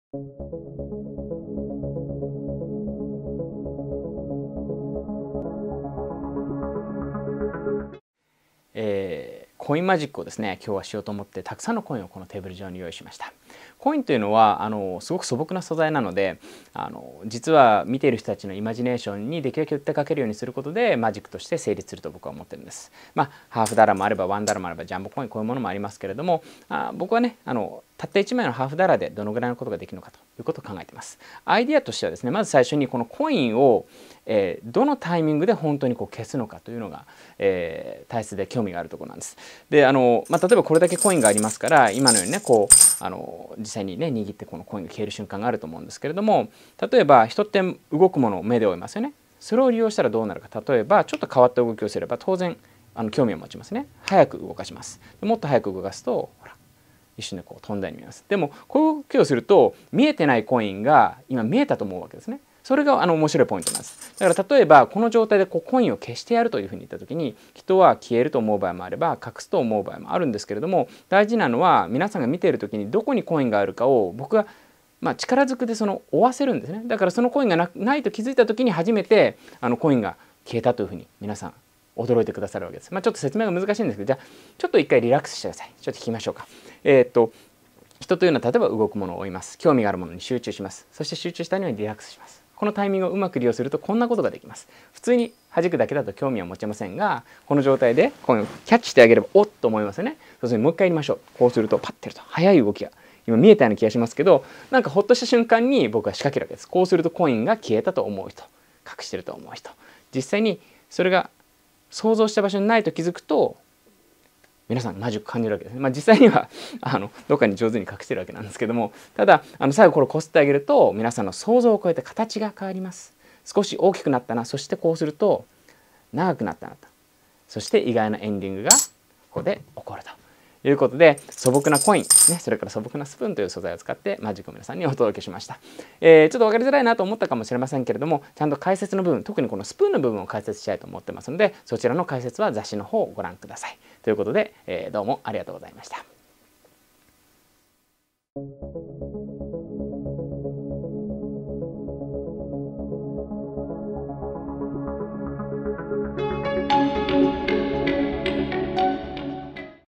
JPMA NEW123誌 Vol 3 2021年秋号 緒川集人 Lecture Act
協会季刊誌1-2-3第３号にコインの名手、緒川集人さんが登場！ワンコインルーティンから、クライマックスの手順を演技披露！
本誌、紙面ではそのクライマックスの手順を詳しくレクチャー解説！

「NEWワン・ツー・スリー」は年４回(春、夏、秋、冬)発行します。1冊1500円
是非年間購読お申し込み下さい。
1年間購読料6000円(税、送料込み)
お申し込み